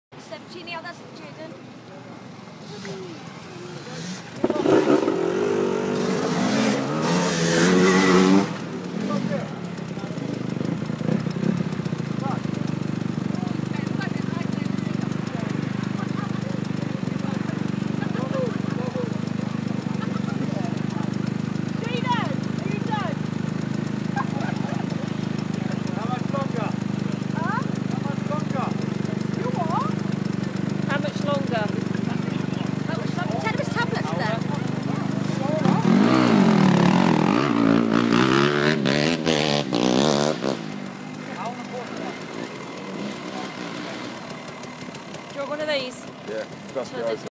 The Old Days of Old Blokes - Hare and Hounds Exhaustion
totally knackered at a hare and hounds race